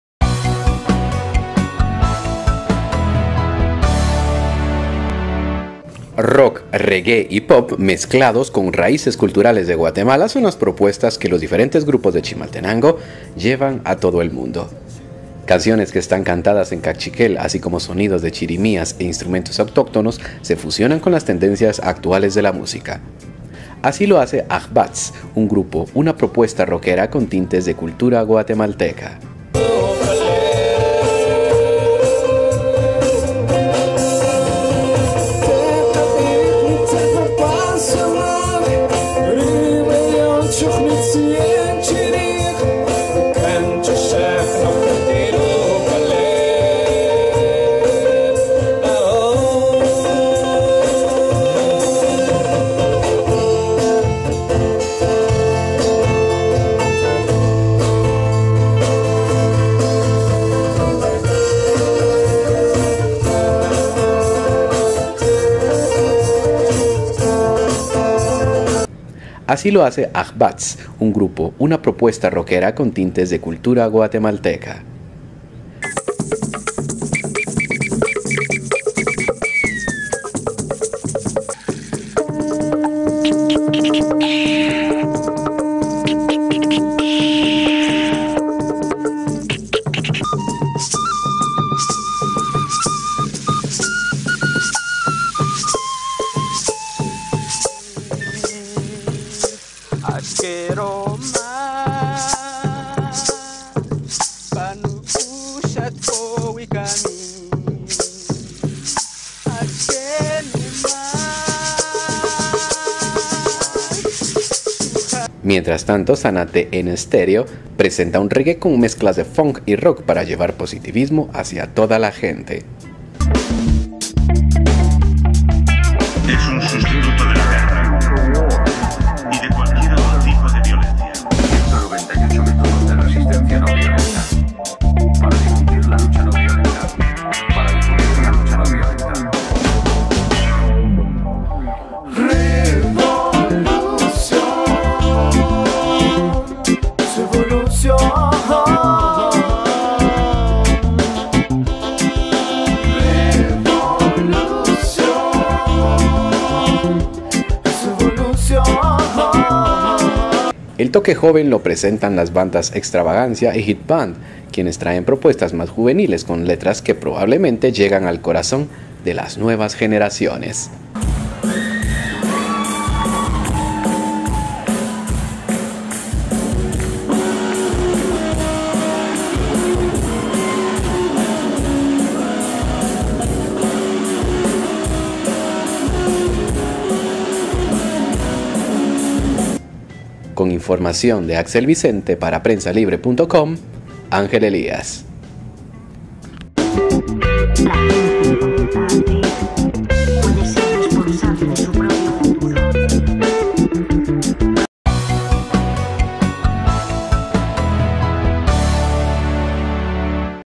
Bandas chimaltecas destacan en el país
Agrupaciones en el departamento de Chimaltenango demuestran su talento musical en distintos festivales y presentaciones

Reporte: Ángel Elías